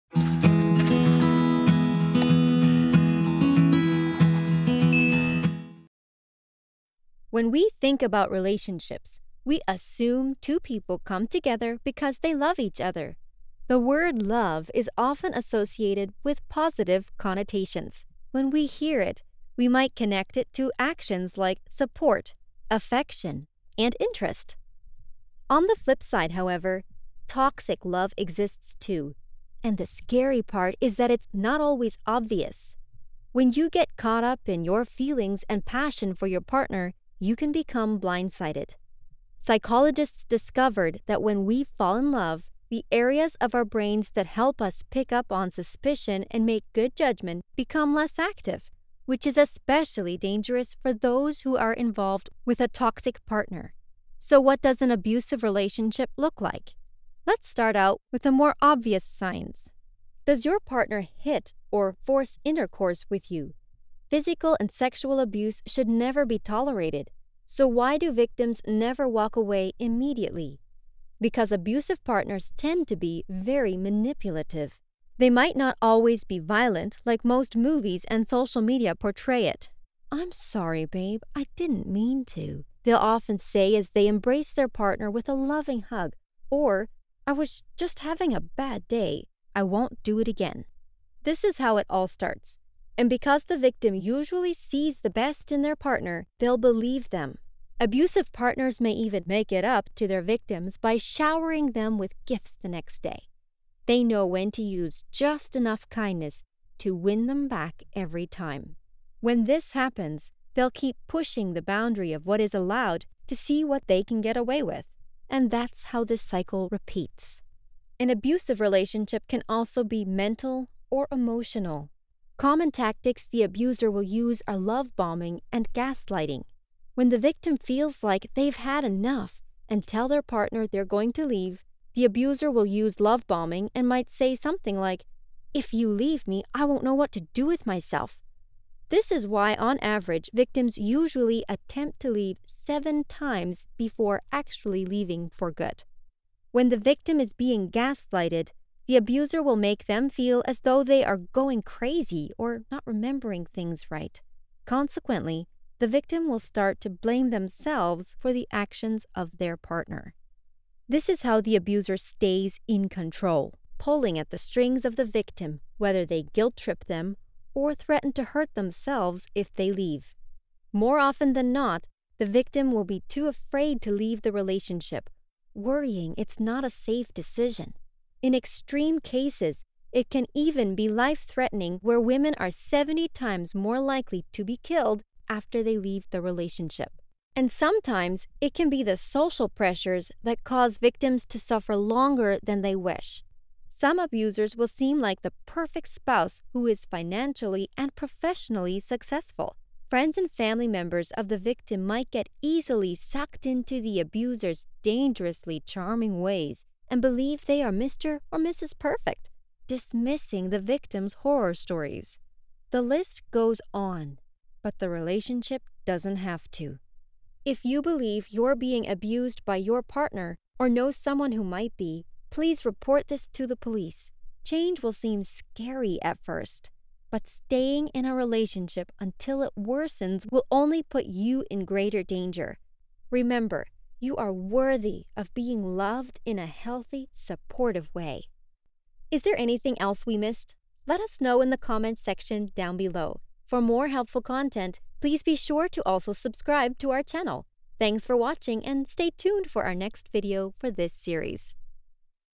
What Does Relationship Abuse Look Like? (Abuse Series)
Does your partner hit or force intercourse with you? Physical and sexual abuse should never be tolerated. So, why do victims never walk away immediately? Because abusive partners tend to be very manipulative. Are you currently in a toxic relationship?

Must be 18+ to sign up.

Credits:
Script: Catherine Huang
Edited by: Steven Wu
Vo: Florence Dueck
Animator: Rachel M.
YouTube Manager: Cindy Cheong

References:

11 Reasons Why People in Abusive Relationships Can’t “Just Leave”. (2018). One Love. Retrieved January 24, 2019.

Feuerman, M. (2018, July 8). 21 Warning Signs of an Emotionally Abusive Relationship. Psych Central. Retrieved January 24, 2019.

Koehn, B. (2017, October 31). Early Signs of an Unhealthy Relationship. HuffPost. Retrieved January 24, 2019.

Praver, F. (2011, August 14). Beware: Love Is Blind. Psychology Today. Retrieved January 24, 2019.